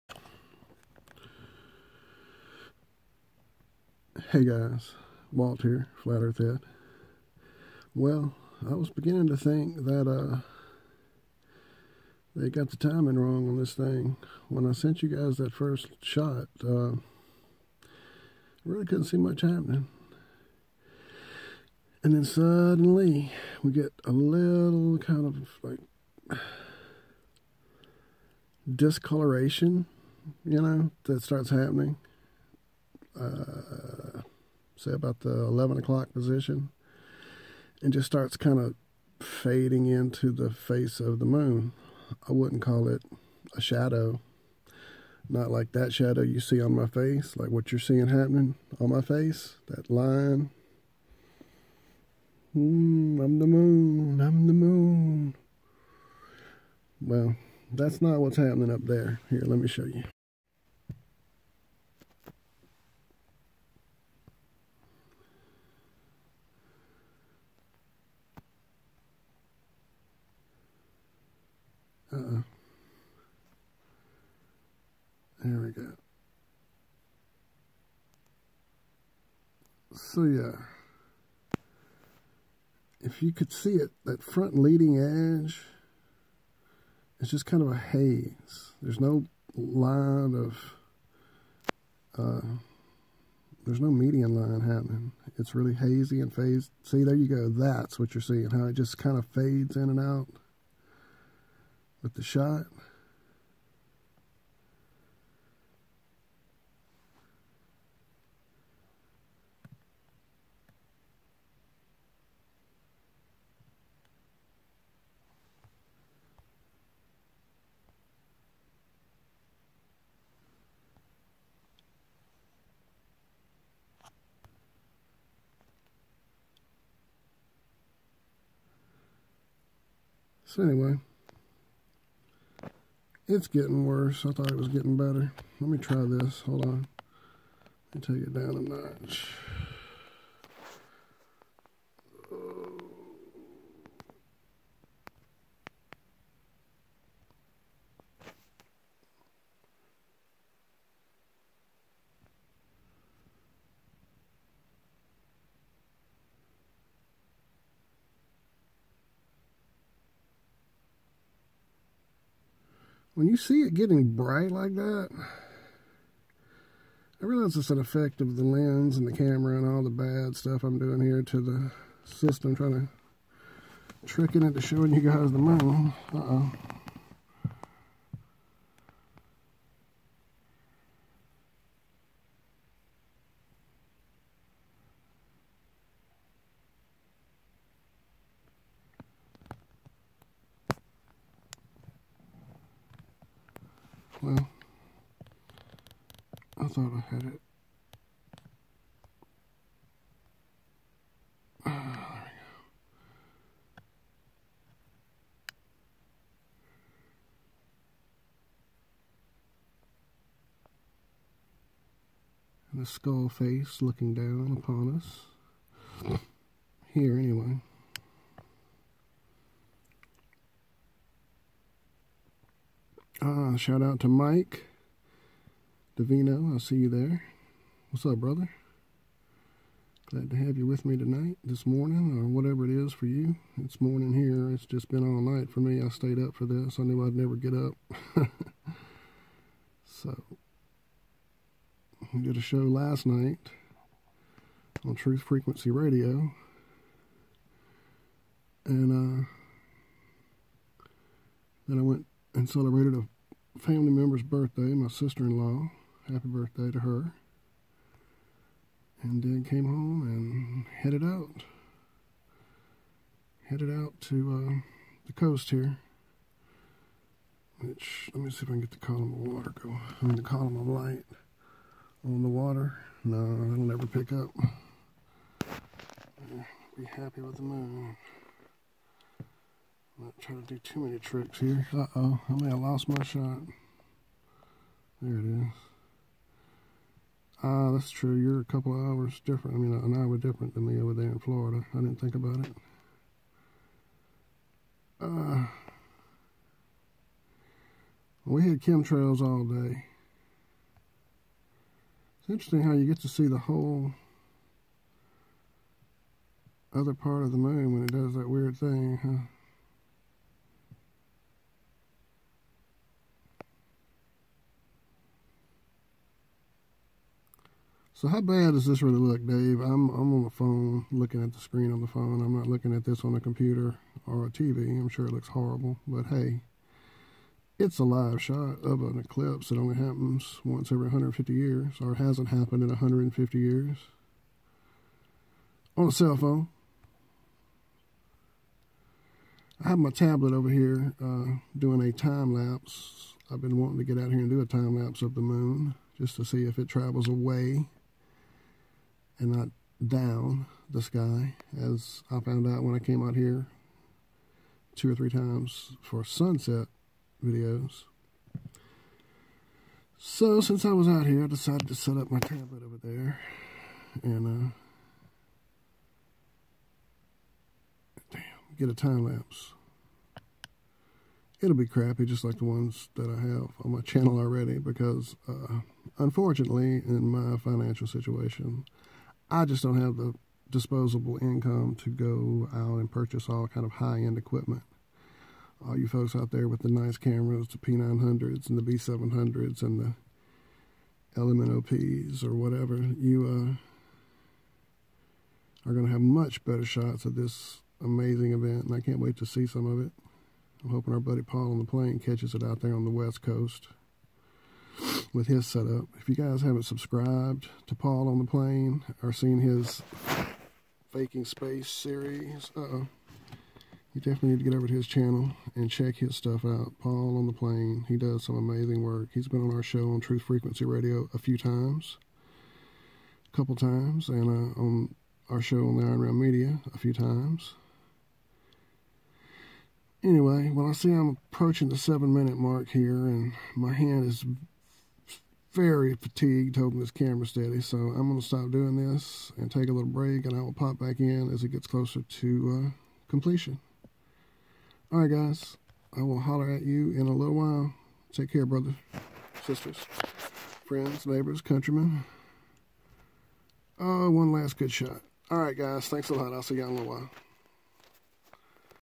Super Blue Blood Moon Eclipse Jan 2018 Pt2 (Cleaned Up)
I used the YT video editor to try and remove some of the shaky camera issues...If it is still crap, Oh well.